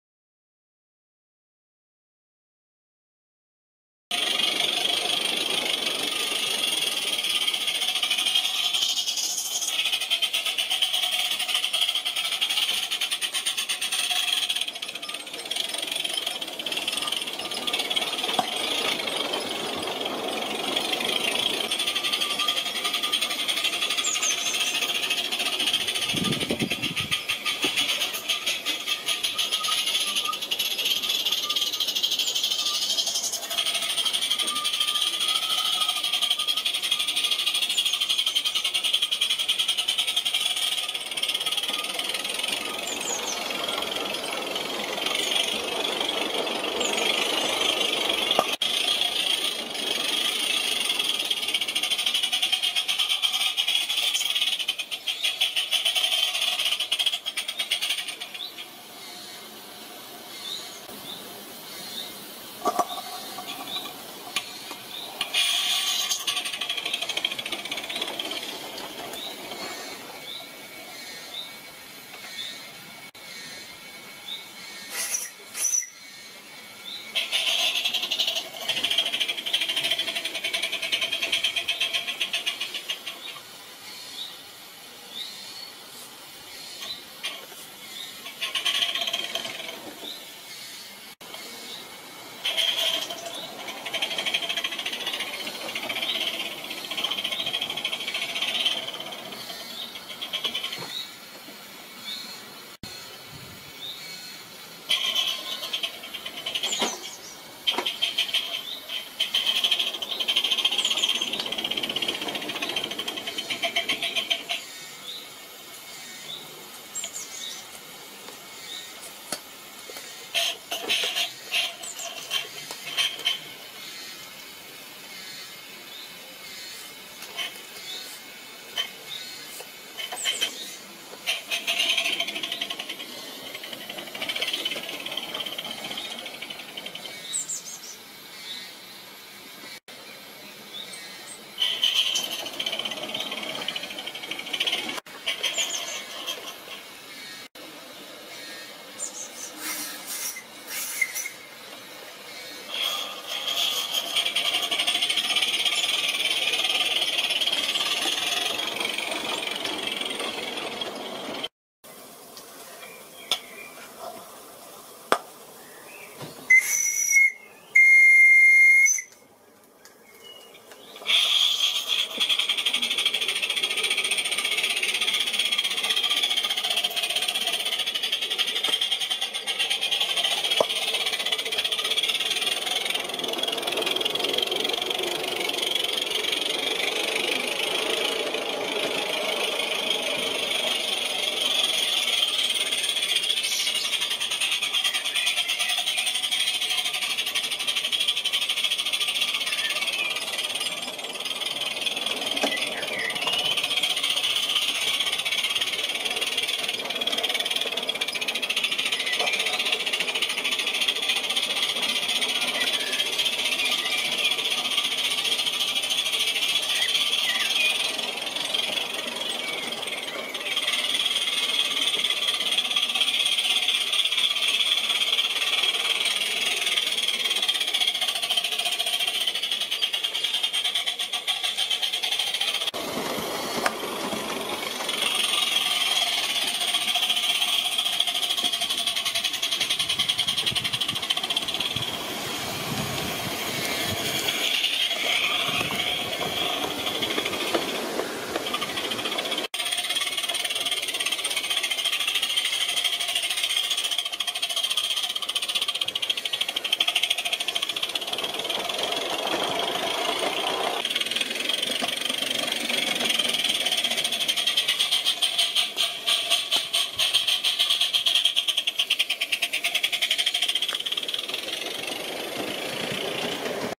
Live Steam on the balcony
In this video I am running my modified Cheddar Models Iver on a temporary railway set up on the balcony.